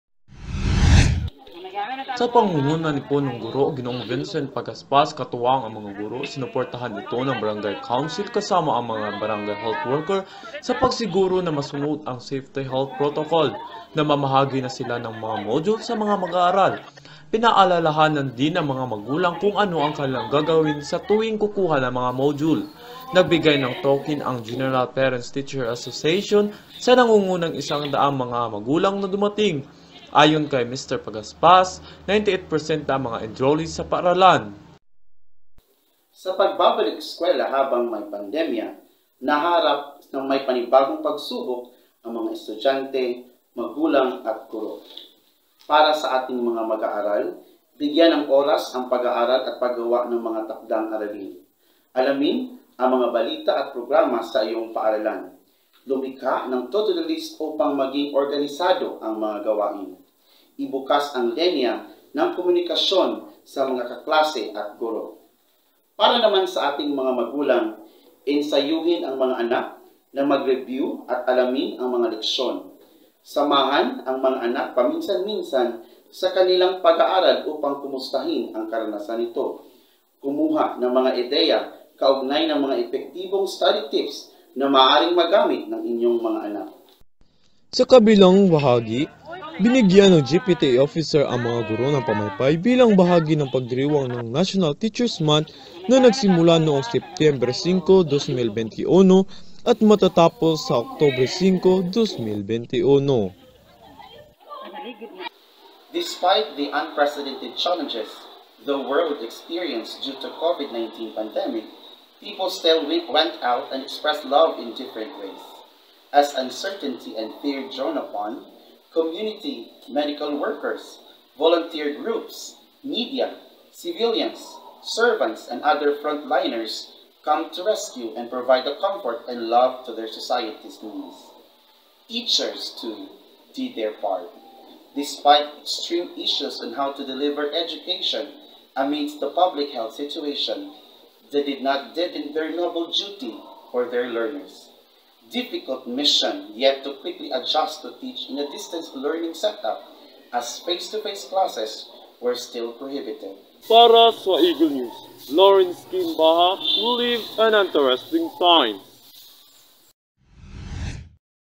Oplan Balik Eskwela 2021, inilunsad sa Tinagaan Integrated School sa Salay, Misamis Oriental
Naglunsad ng Oplan Balik Eskwela 2021 ang Tinagaan Integrated School sa Brgy. Tinagaan, Salay, Misamis Oriental sa pangunguna ni G. Vincent Pagaspas, punungguro, katuwang ang mga guro ng nasabing paaralan. (Laurence Kim Baja)